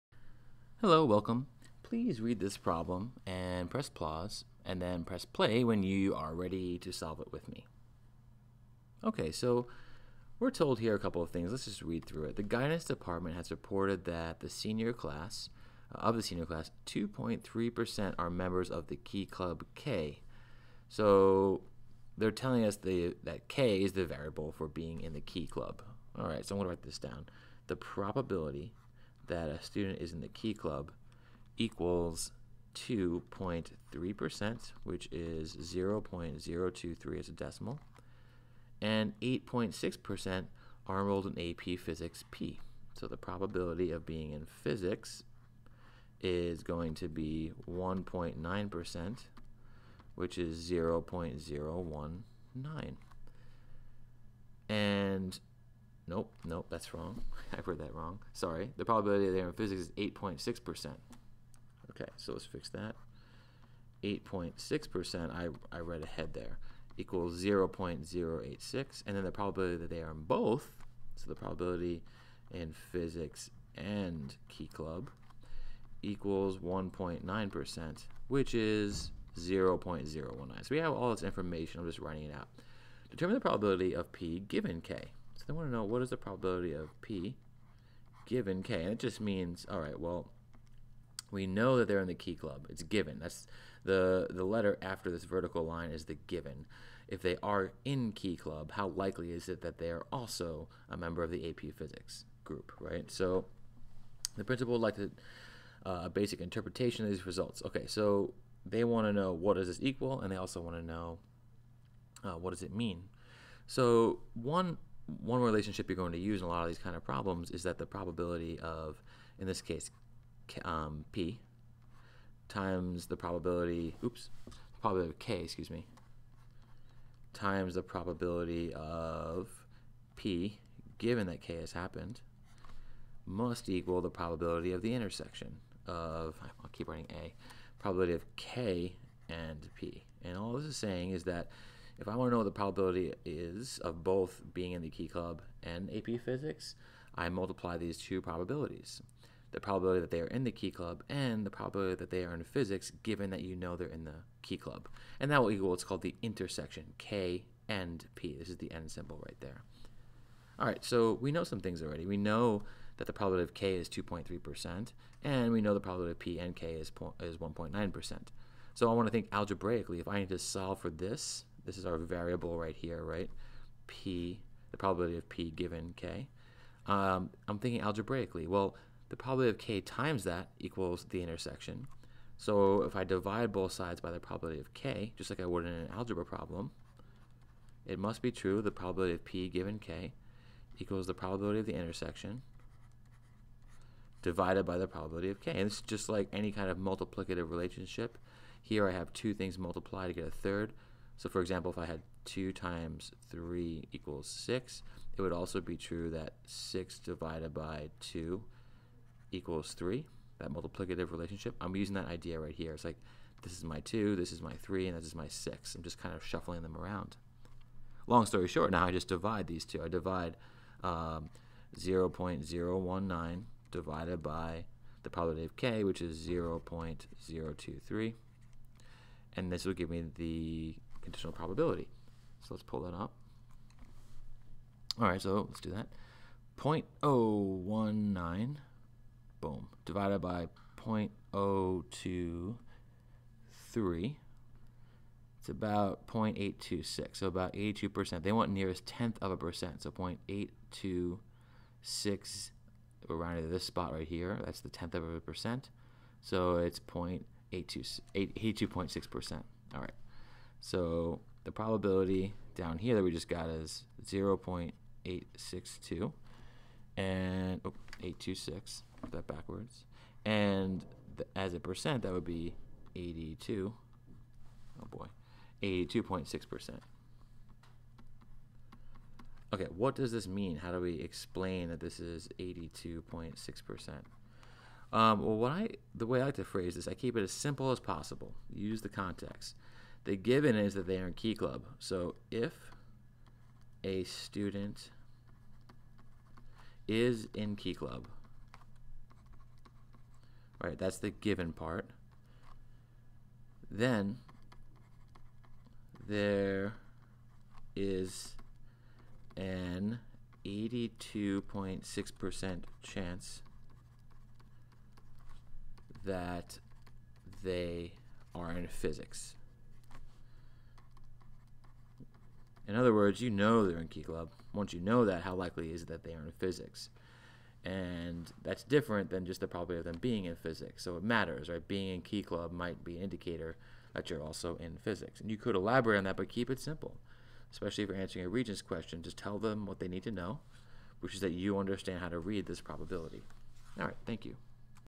Algebra 2 Regents January 2017 #35
In this video, we work through a probability example from the January 2017 Algebra 2 Regents exam.